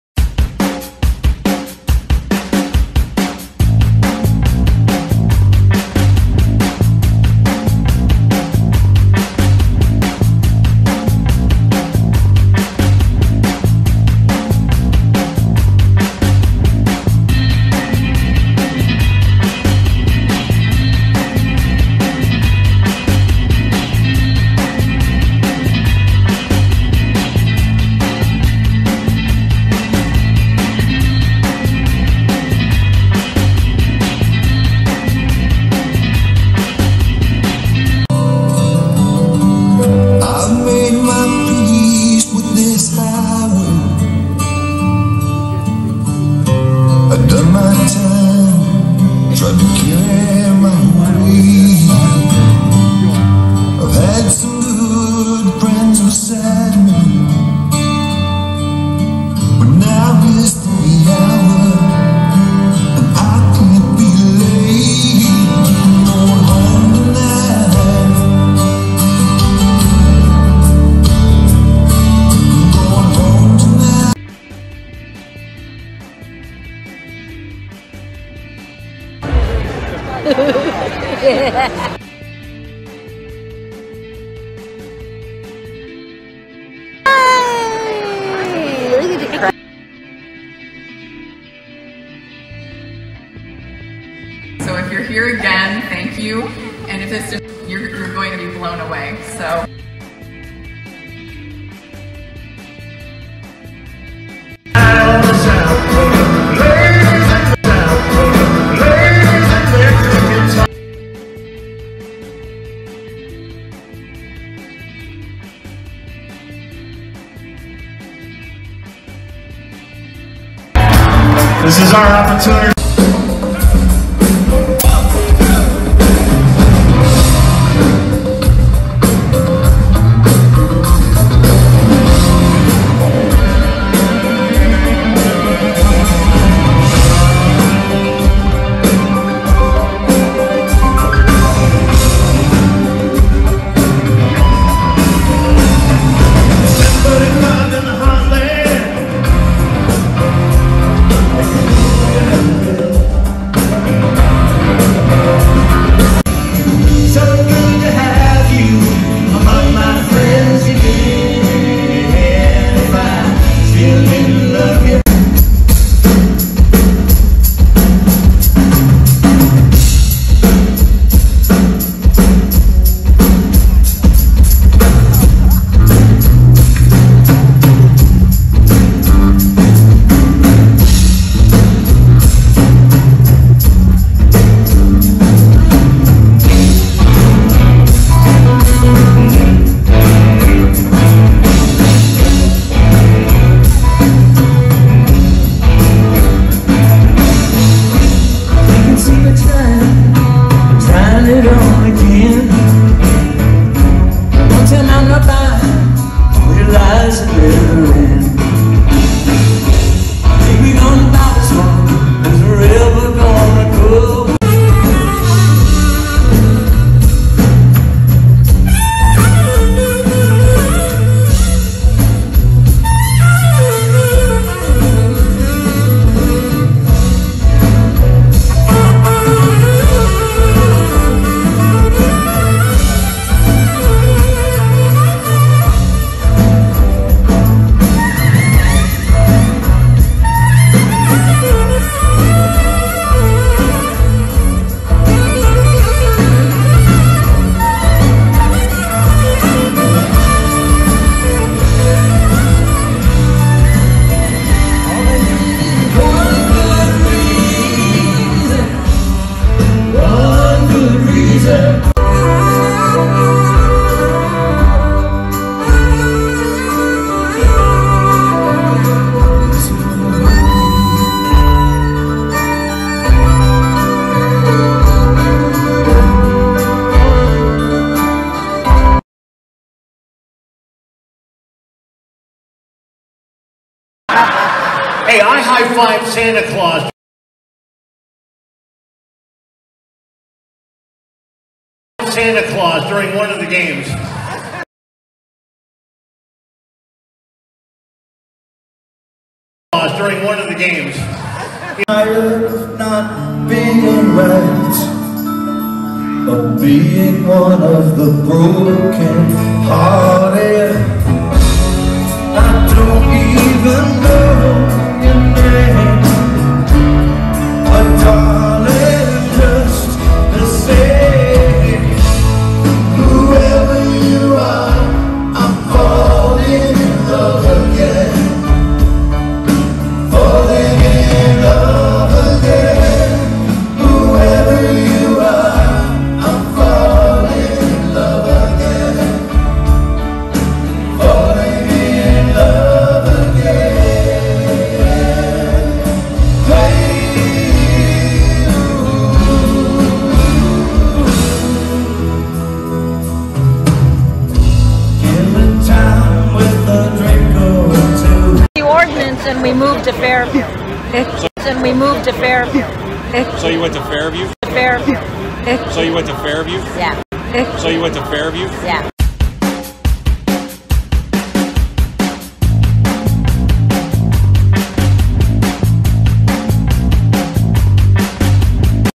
Life with Me Gina Marie Tribute Concert
Life with Me Gina Marie

This is just some snippets of photos and videos
From the concert at the MGM in Northfield Ohio
with my good friend.

The Michael Stanley
Among Friends Again Concert